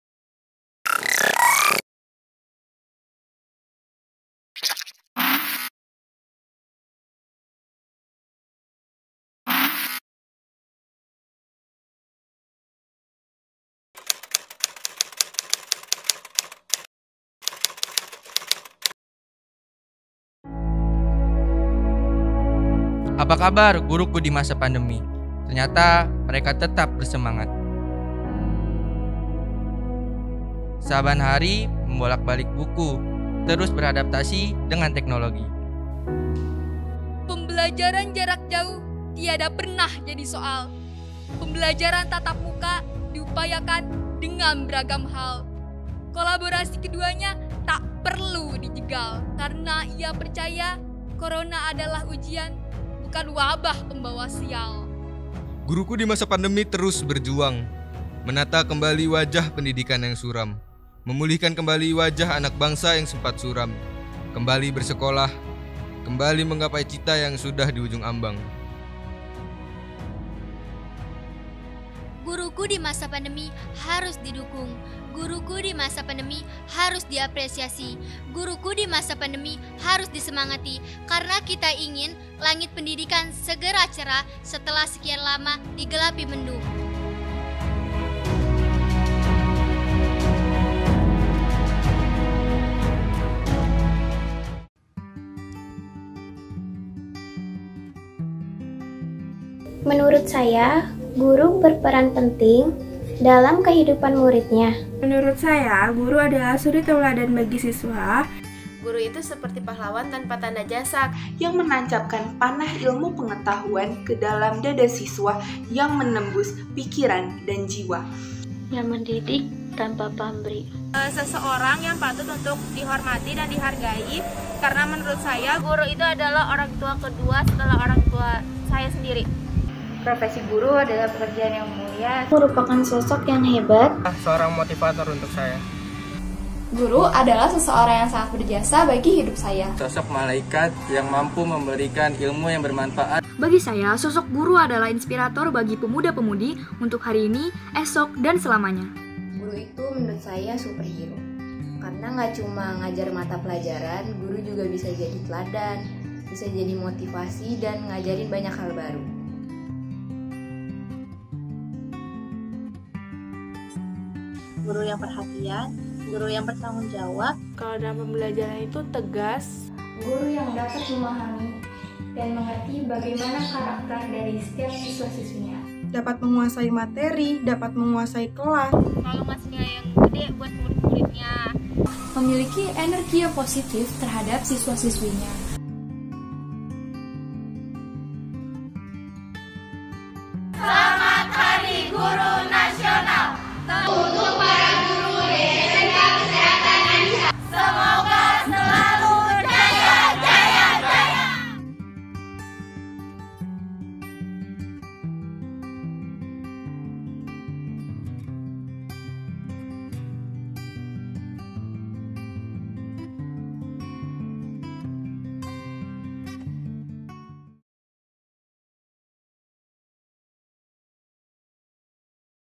SELAMAT HARI GURU NASIONAL dari SISWA/I SMK KESEHATAN ANNISA
Ucapan Selamat Hari Guru Nasional untuk Guru
dari siswa/i SMK Kesehatan Annisa
Program Keahlian Farmasi dan Tata Kecantikan